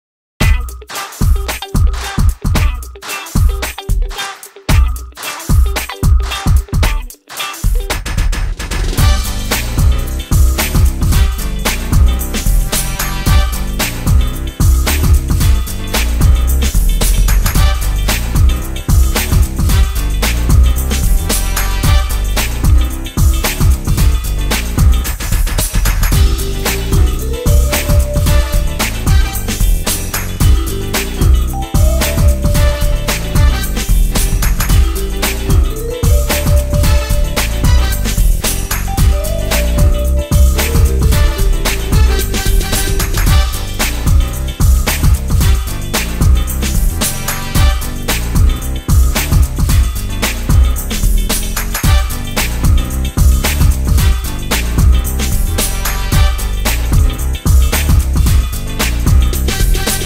First StopMotion Video- Danglet for iPod & iPhones
Unboxing of the Danglet sent to me for review by CollinsAmerica:
The Danglet- Wrist and Neck Strap for ipod and iphone.